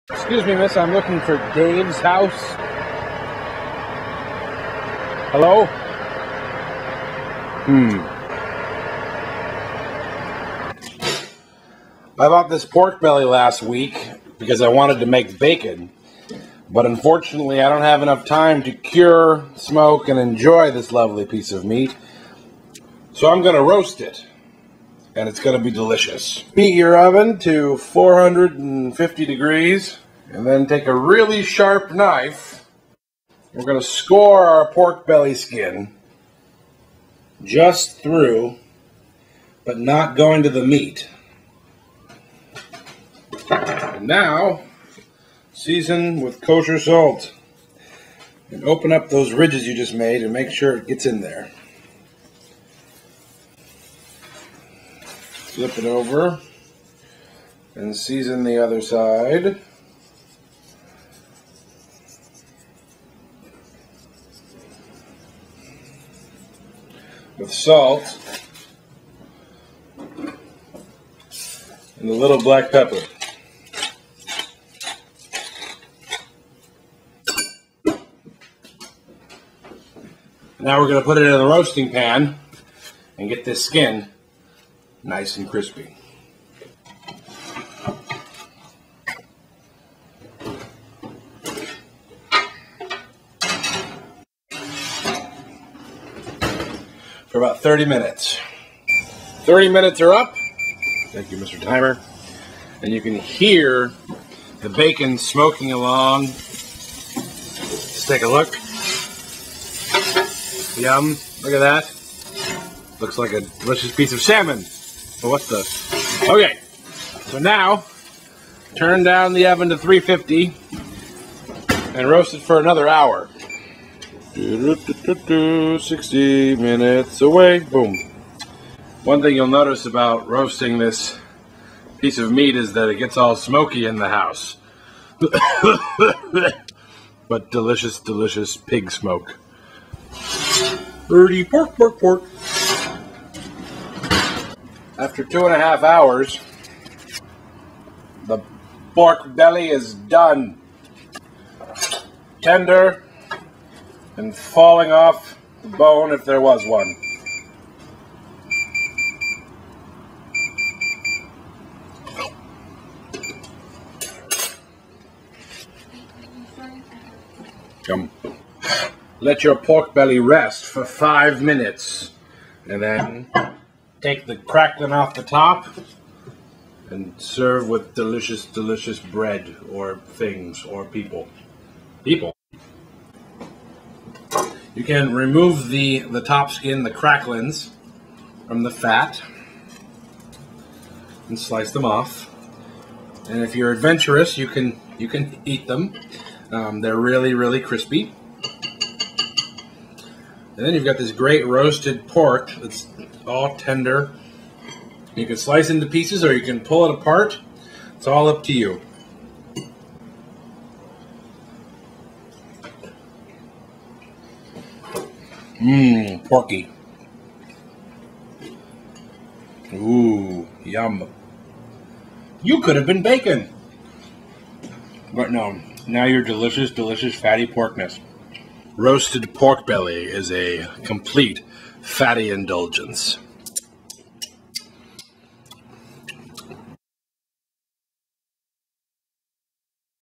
Roasted Pork Belly
Alex has a delicious fat covered roast of pig.